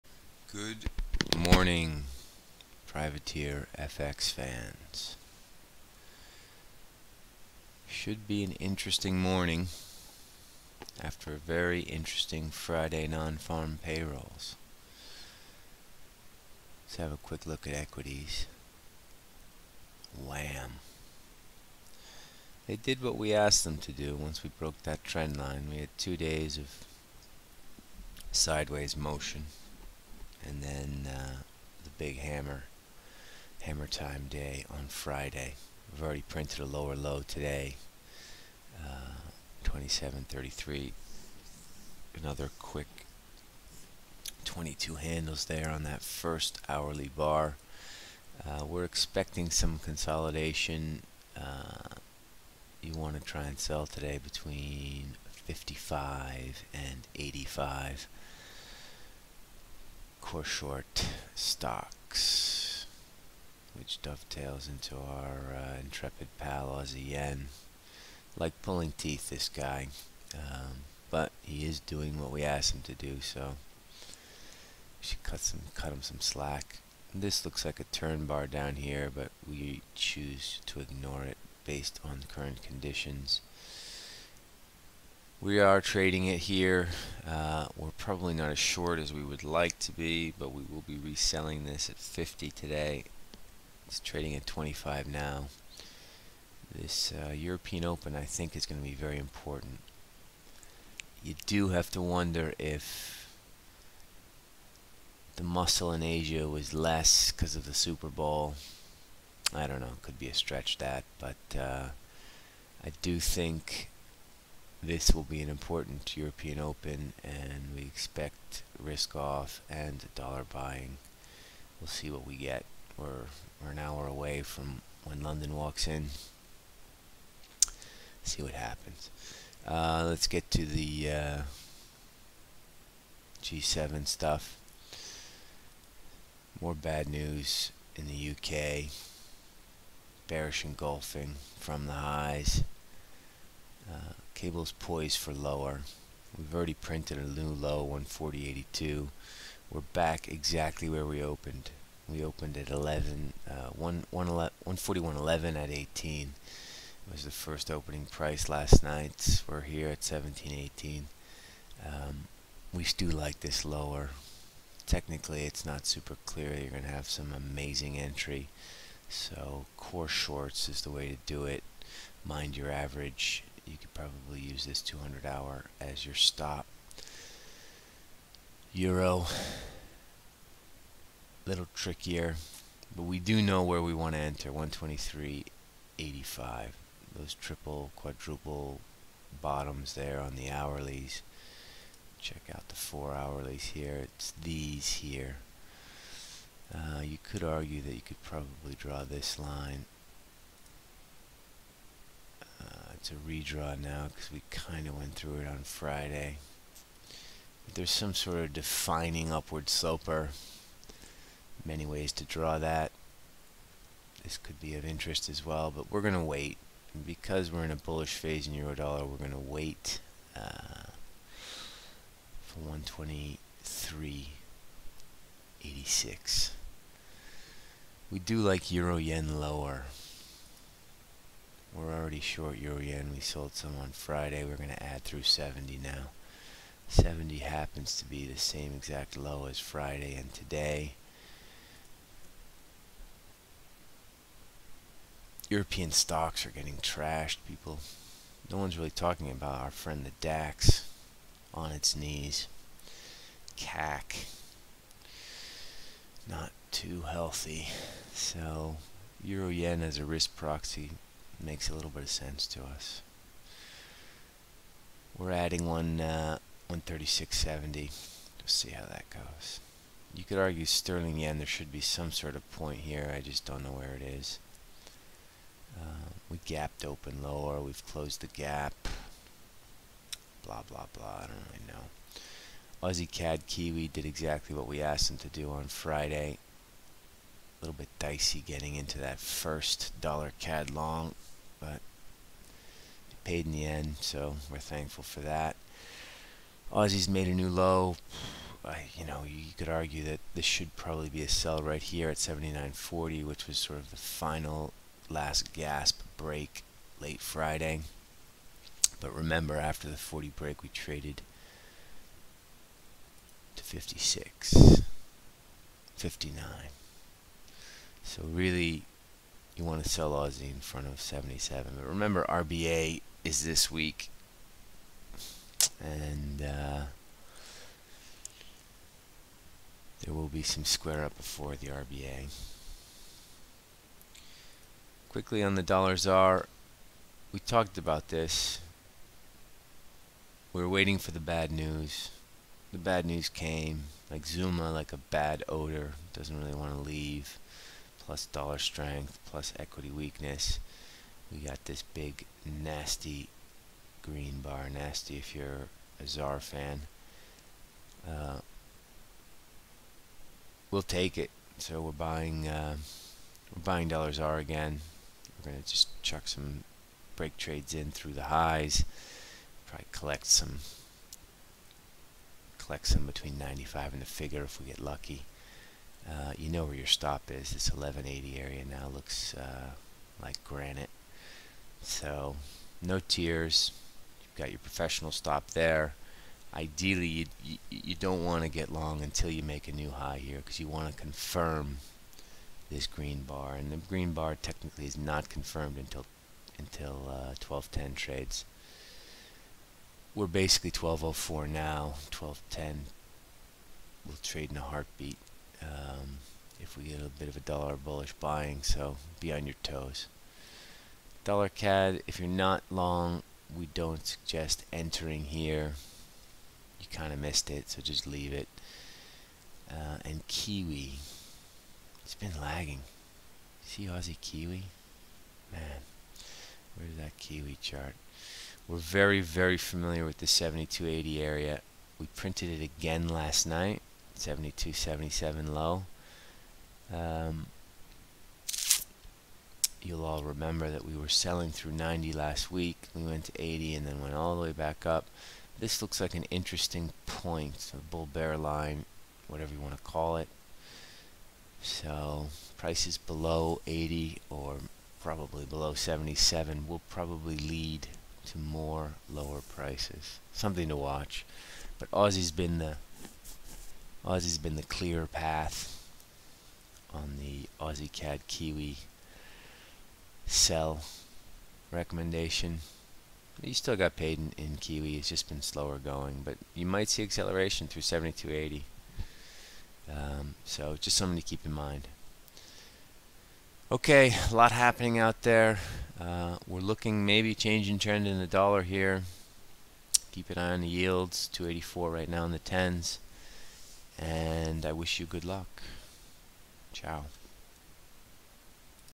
5Feb18 Europe Opening Call - PrivateerFX
European opening important today, we expect RiskOff & USD buying

Equities
NFP gone wham on SPX core short for us
DAX CAC getting trashed

FX SetUps
AUSJPY EURJPY AUD CAD NZD USDZAR
GBPUSD EURUSD

YouTube subs for earliest view of vids

Web: PrivateerFX.com

THIS WAS
5Feb18 Europe Opening Call - PrivateerFX